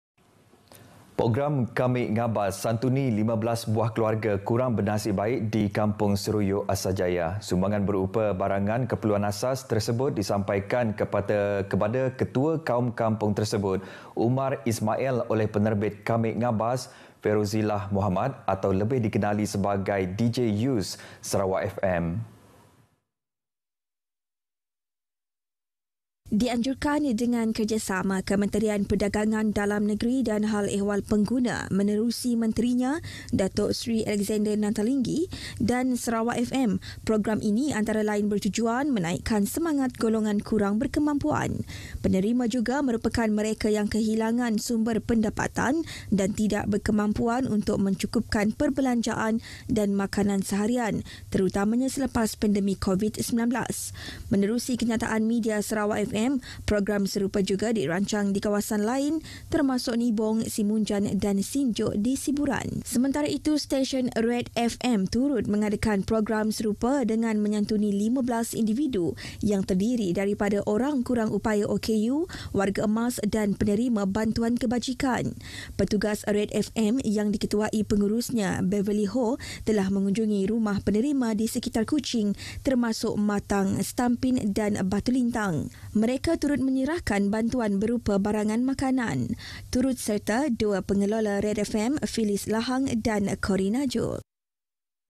PROGRAM " KAMEK NGABAS"
Today’s SARAWAKfm activities Kamek Ngabas
RTM-KPDNHEP programme.
This is a charity programme to help those in need.❣️

AHAD | 14 OGOS 2022 | KPG SERUYUK